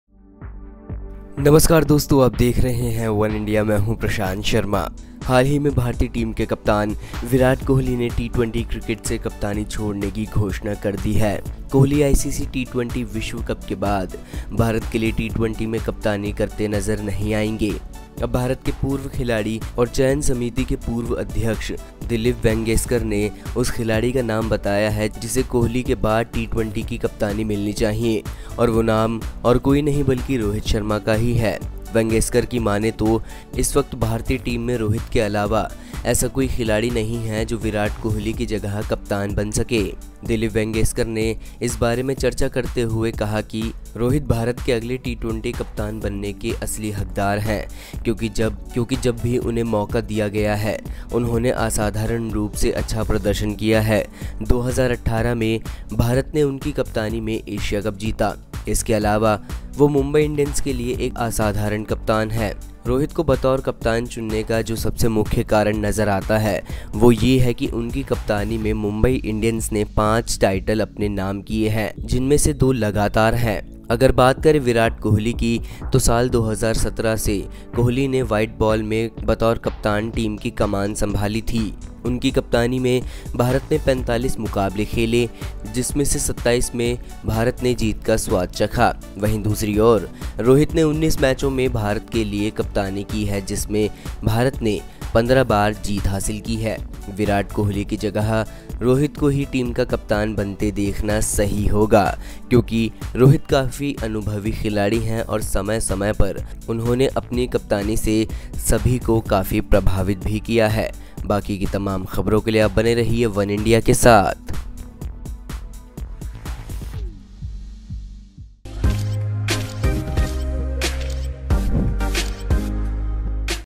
T20 Captaincy: Dilip Vengsarkar choose Rohit Sharma as the new India t20 Captain | Oneindia Sports
Recently, Indian team captain Virat Kohli has announced his retirement from T20 cricket. Kohli will not be seen captaining India in T20s after the ICC T20 World Cup. Now former India player and former selection committee chairman Dilip Vengsarkar has named the player who should be given the captaincy of Kohli's T20 and that name is none other than Rohit Sharma.According to Vengsakar, at this time there is no other player in the Indian team other than Rohit Aisa who can become captain in place of Virat Kohli.

हालही में भारतीय टीम के कप्तान विराट कोहली ने टी20 क्रिकेट से कप्तानी छोड़ने की घोषणा कर दी है। कोहली ICC टी20 विश्व कप के बाद भारत के लिए टी20 में कप्तानी करते नज़र नहीं आएंगे। अब भारत के पूर्व खिलाड़ी और पूर्व चयन समिति के पूर्व अध्यक्ष दिलीप वेंगसरकर ने उस खिलाड़ी का नाम बताया है जिसे कोहली के टी20 की कप्तानी मिलनी चाहिए और वो नाम और कोई नहीं बल्कि रोहित शर्मा का ही है। वेंगसकर की माने तो इस वक़्त भारतीय टीम में रोहित ऐसा के अलावा ऐसा कोई खिलाड़ी नहीं है जो विराट कोहली की जगह कप्तान बन सकें।